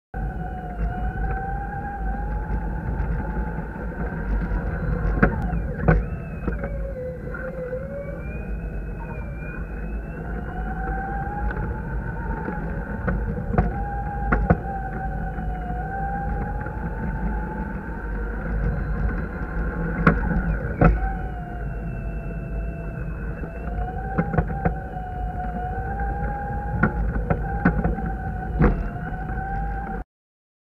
Flashkart 28.095
Szülinapomra kaptam egy kis flashkartozást, első alkalomra lett 28.095 az időm. Lesz ez még jobb is! ;)
Ajánlom mindenkinek kipróbálásra, egyáltalán nem rosszabb, mint a robbanómotoros verzió!